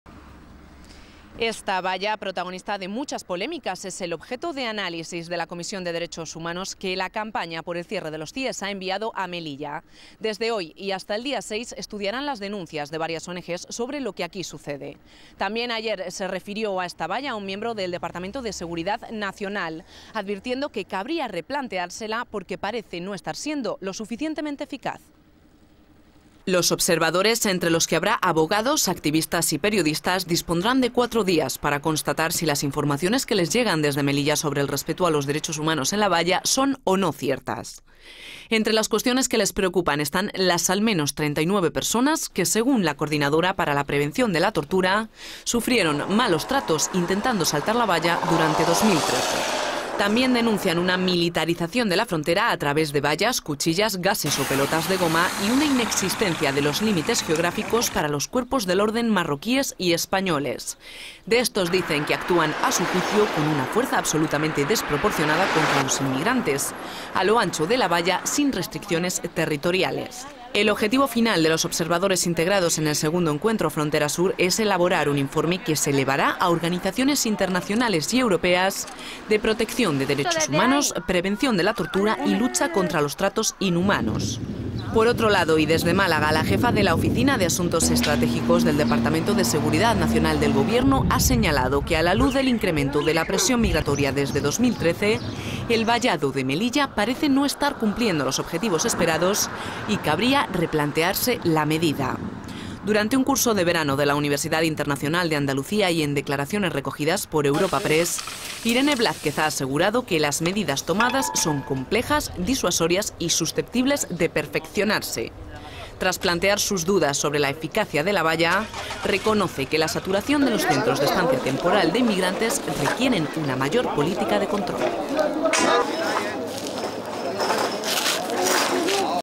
Durante tres días una comisión de observadores de Derechos Humanos visita Melilla
Desde hoy y durante tres días, una comisión de observadores de Derechos Humanos constituida por el movimiento "Campaña por el cierre de los CIES" visita Melilla. Lo hace en el marco del segundo Encuentro Frontera Sur y su intención es estudiar la situación en el perímetro fronterizo y determinar si se vulneran o no los derechos humanos en ella y en su entorno. Además, voces del Departamento de Seguridad Nacional creen que la valla no está cumpliendo su objetivo y habría que replanteársela.

Esta valla, siempre polémica, es el objeto de análisis de la comisión de Derechos Humanos que la "Campaña por el cierre de los CIES" ha trasladado a Melilla. Desde hoy y hasta el día 6, estudiarán  las denuncias de las oenegés sobre lo que aquí sucede. También se ha referido a esta valla un miembro del Departamento de Seguridad Nacional, advirtiendo que cabría replanteársela porque "parece no estar cumpliendo los objetivos esperados".

Los observadores, entre los que habrá abogados, activistas y periodistas, dispondrán de cuatro días para constatar si las informaciones que les llegan desde Melilla sobre el respeto a los Derechos Humanos en la valla son o no ciertas. Entre las cuestiones que les preocupan están las al menos 39 personas que, según la Coordinadora para la Prevención de la Tortura, sufrieron malos tratos intentando saltar la valla durante 2013.

También denuncian una "militarización de la frontera" a través de vallas, cuchillas, gases o pelotas de goma, y una inexistencia de los límites geográficos para los cuerpos del orden marroquíes y españoles, que actúan, a su juicio, "con una fuerza absolutamente desproporcionada" contra los inmigrantes, a lo ancho de la valla sin restricciones territoriales.

El objetivo final de los observadores integrados en el segundo Encuentro Frontera Sur es elaborar un informe que se elevará a organizaciones internaciones y europeas de protección de derechos humanos, prevención de la tortura y lucha contra los tratos inhumanos.

Por otro lado, y desde Málaga, la jefa de la oficina de Asuntos Estratégicos del Departamento de Seguridad Nacional del Gobierno ha señalado que, a la luz del incremento de la presión migratoria desde 2013, "el vallado de Melilla parece no estar cumpliendo los objetivos esperados y cabría replantearse la medida". Durante un curso de verano de la Universidad Internacional de Andalucía, y en declaraciones recogidas por Europa Press, Irene Blázquez ha asegurado que "las medidas tomadas son complejas, disuasorias" y susceptibles de perfeccionarse. Tras plantear sus dudas sobre la eficacia de la valla, reconoce que la saturación de los Centros de Estancia Temporal de Inmigrantes requieren una mayor política de control.